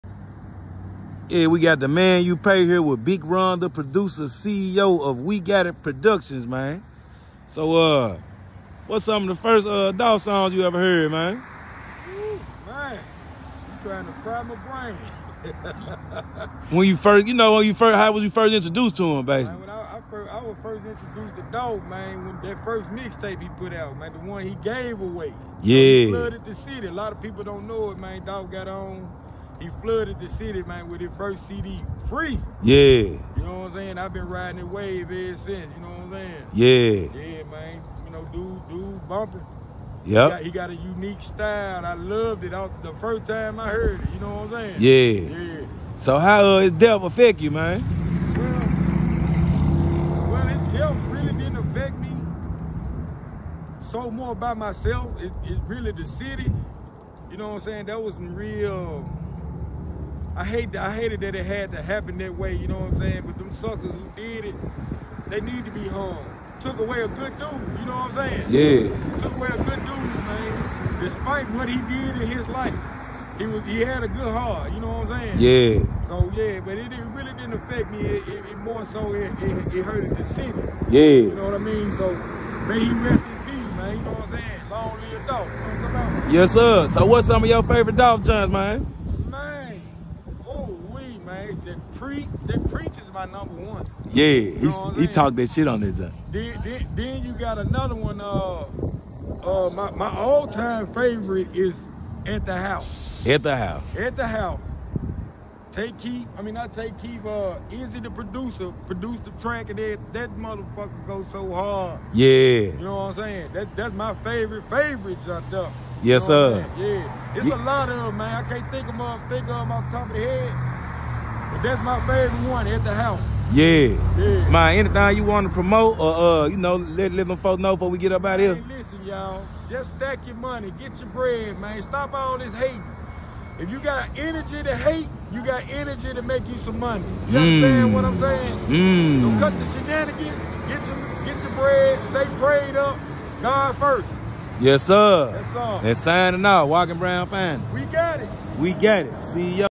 Big Run the Producer C.E.O of W.G.I give’s his opinion on Young Dolph assassin in Memphis Tennessee
Interview produced on 1/28/22